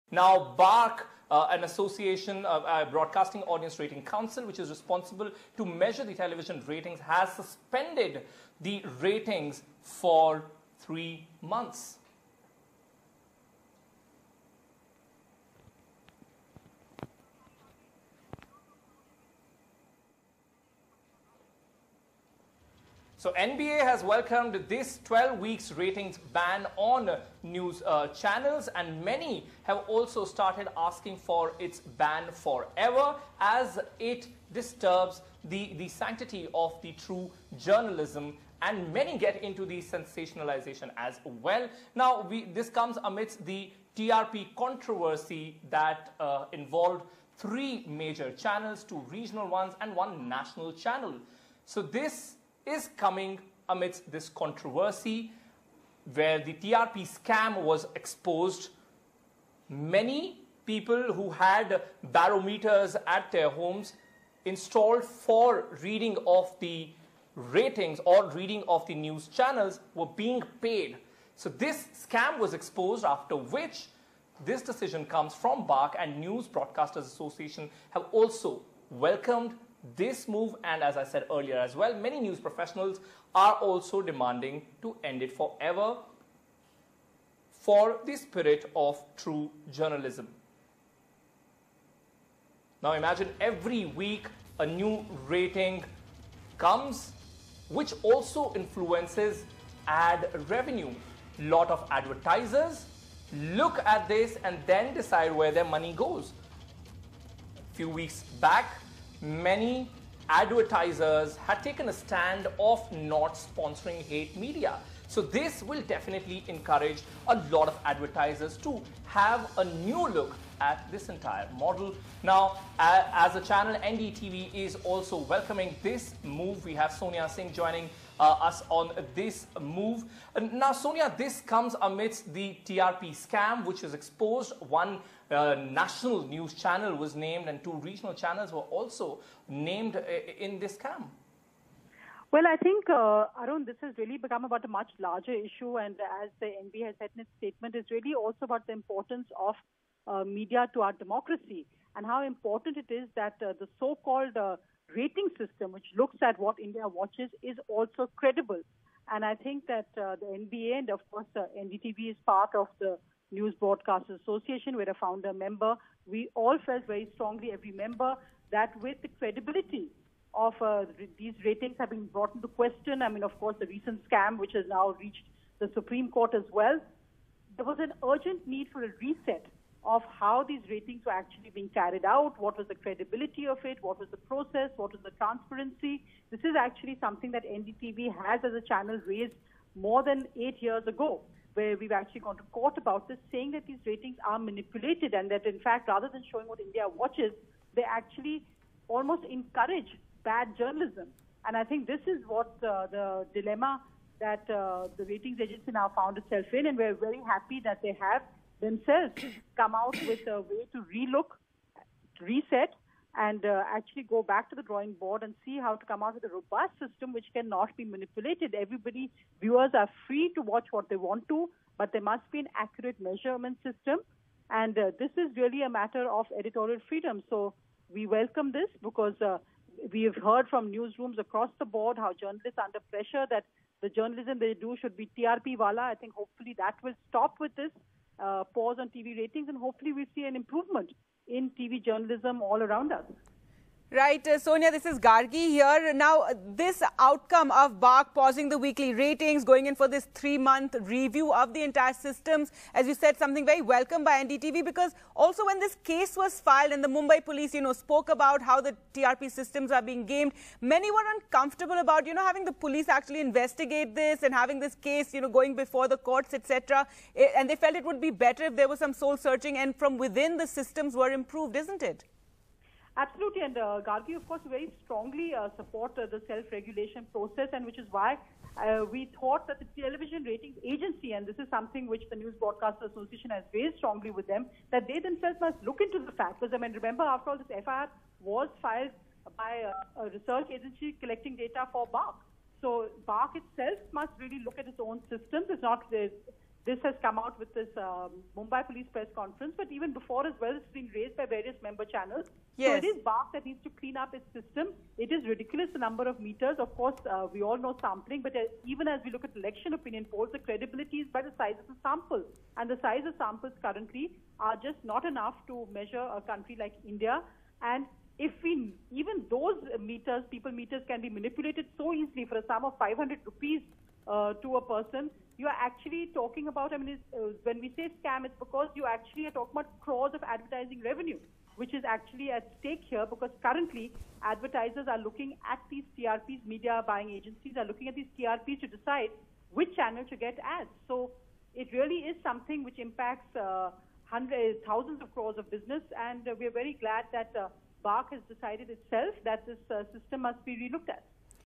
BARC TV Ratings: Agency To Pause News Channel Ratings To Review System
TRP Manipulation: Ratings agency BARC (Broadcast Audience Research Council) has decided to pause weekly ratings for news channels for three months as it reviews its system amid the row over alleged rigging of viewership ratings by three channels.